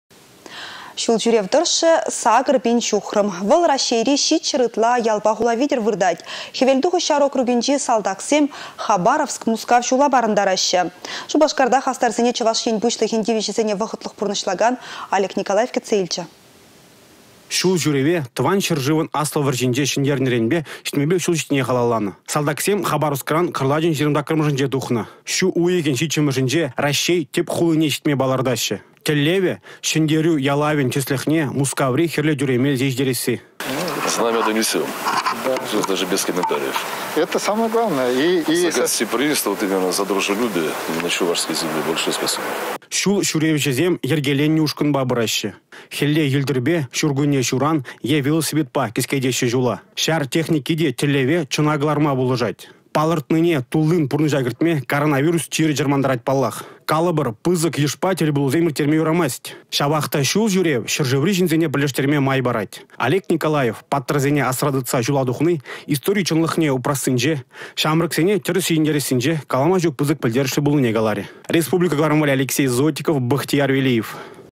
Ҫӗнтерӳ ялавне çитермелле
Çул çӳрев тăршшĕ 8 пин çухрăм. Вăл Раççейри  700 ытла ялпа хула витĕр выртать. Хӗвелтухӑҫ ҫар округӗнчи салтаксем Хабаровск-Мускав çула парăнтараççĕ. Шупашкарта хастарсене Чăваш Ен Пуçлăхĕн тивĕçĕсене вăхăтлăх пурнăçлакан Олег Николаев кĕтсе илчĕ.